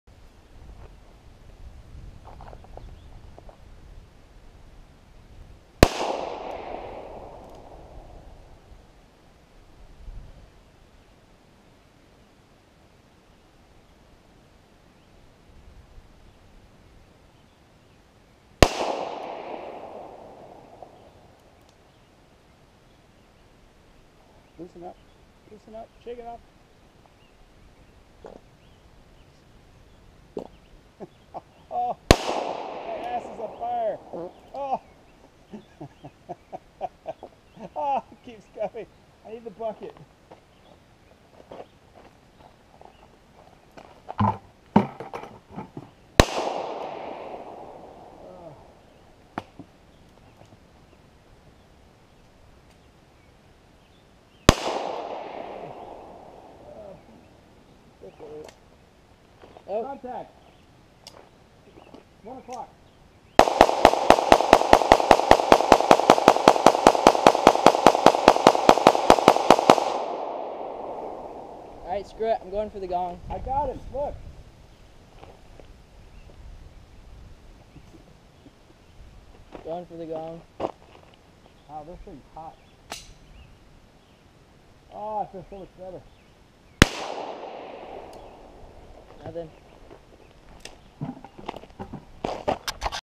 Shelby trying to split a playing card while dad goofs around...
Fun time with family.